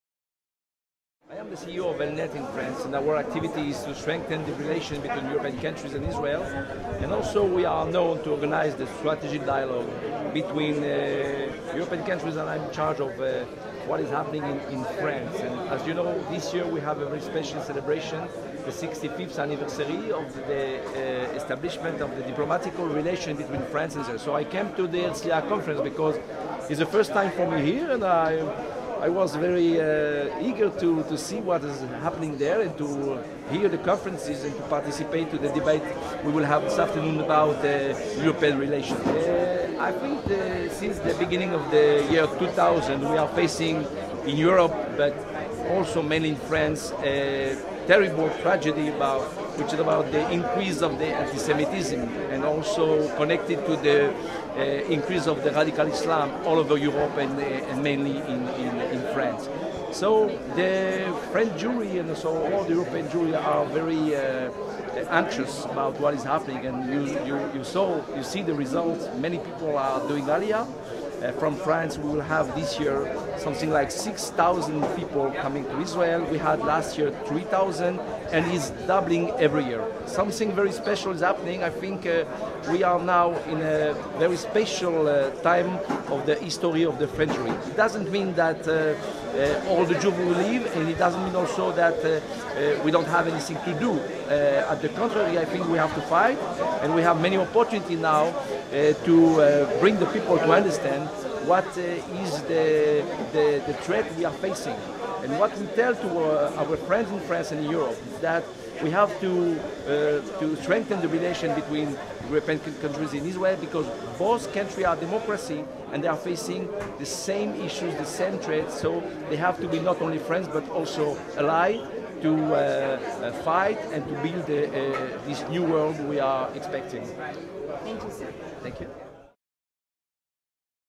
Arie Bensemhoun @ Herzliya Conference
Arie Bensemhoun - CEO of Airnet and the head of the jewish community in Toulouse, France.